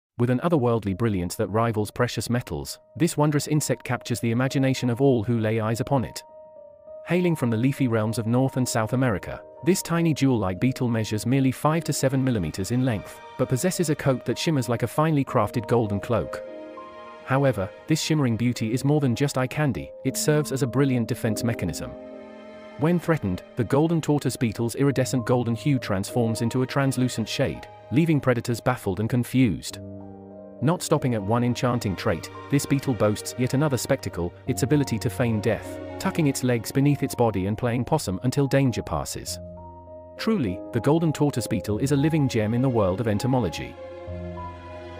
The Golden Tortoise Beetle - Nature's Marvel
The Golden Tortoise Beetle! With an otherworldly brilliance that rivals precious metals, this wondrous insect, scientifically known as Charidotella sexpunctata, captures the imagination of all who lay eyes upon it. Hailing from the leafy realms of North and South America, this tiny jewel-like beetle measures merely 5-7 mm in length but possesses a coat that shimmers like a finely crafted golden cloak. However, this shimmering beauty is more than just eye candy; it serves as a brilliant defense mechanism. When threatened, the Golden Tortoise Beetle's iridescent golden hue transforms into a translucent shade, leaving predators baffled and confused. Not stopping at one enchanting trait, this beetle boasts yet another spectacle—its ability to feign death, tucking its legs beneath its body and playing possum until danger passes. Truly, the Golden Tortoise Beetle is a living gem in the world of entomology."